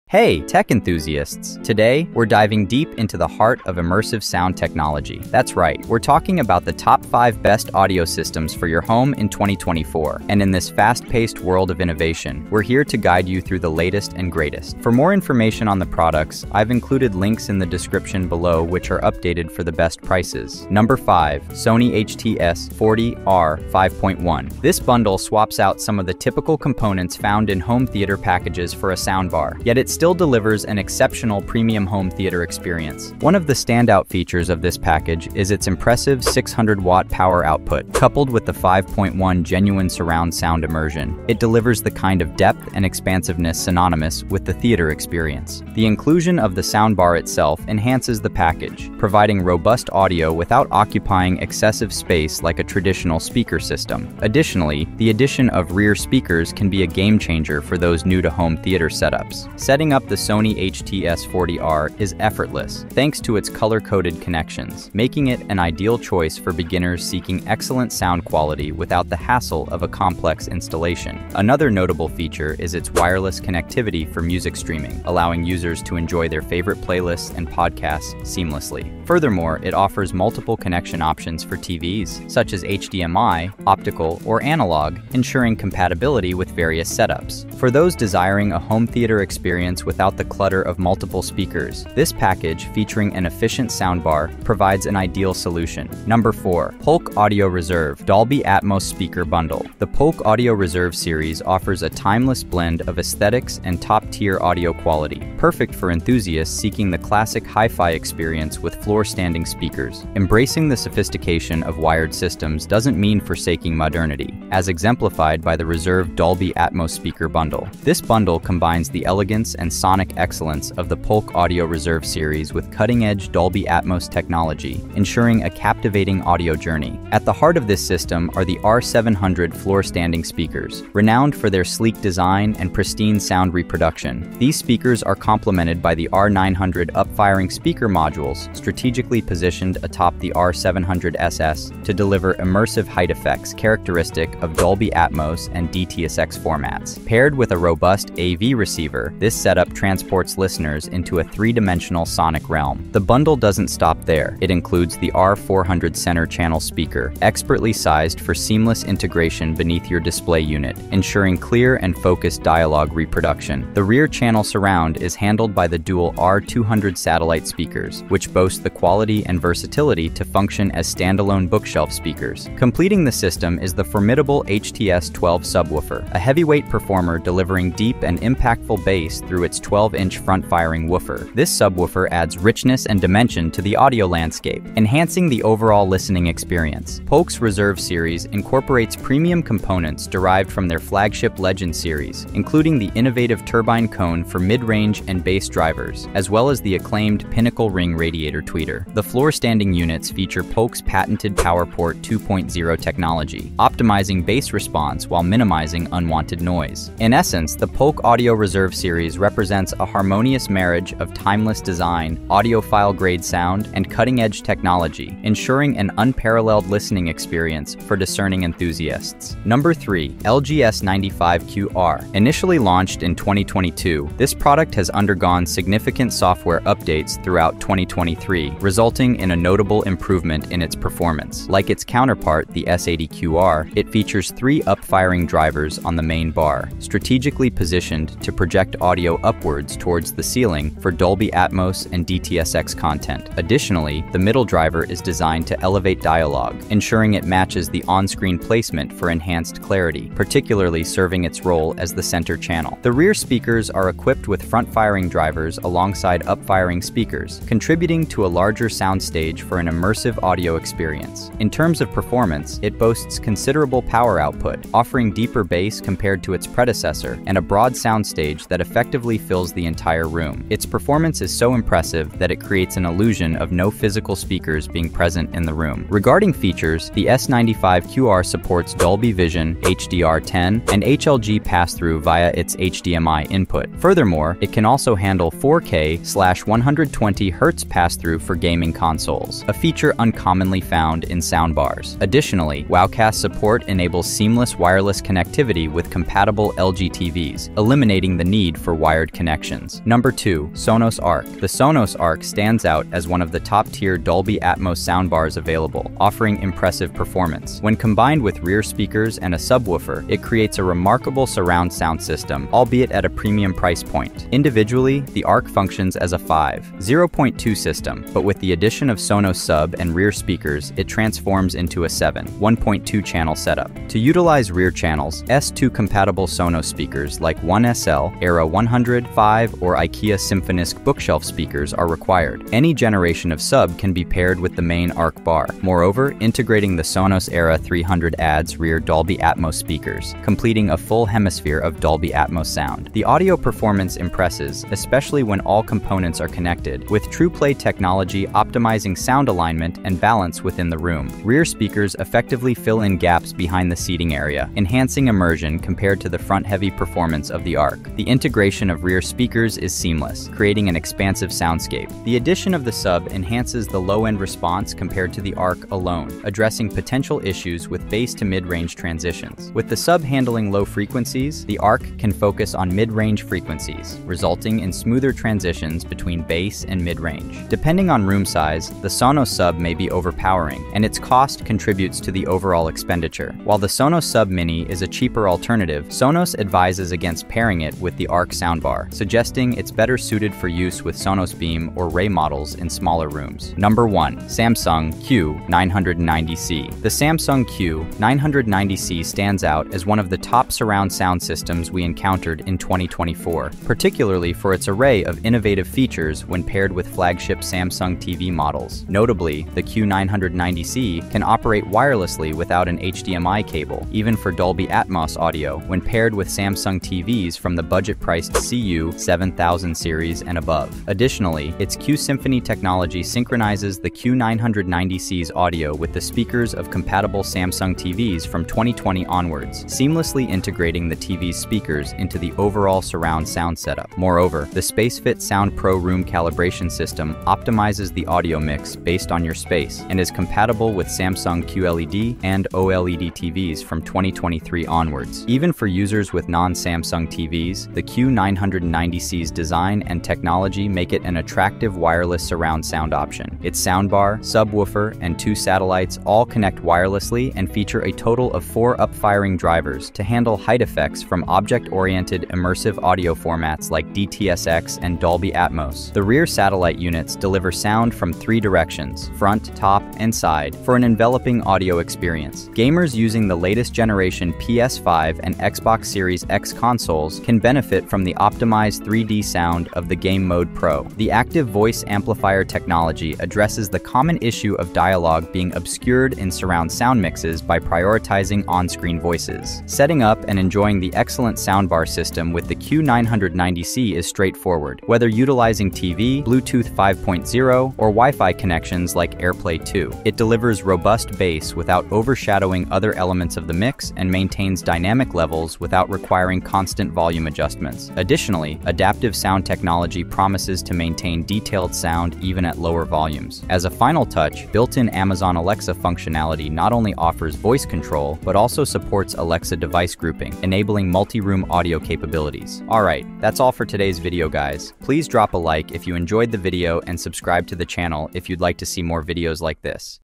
TOP 5 Best Home Theater System 2024
Here are the top 5 best home theater systems in 2024! We’ve made this list for you so you can choose the right one.

➤ Here is the list:
Sony HT-S40R 5.1ch ➤
Polk Audio Reserve Dolby Atmos ➤
LG S95QR ➤
Sonos Arc ➤
Samsung Q990C ➤

Techolist is a participant in the Amazon Services LLC Associates Program. I earn through Amazon associates from qualifying purchases.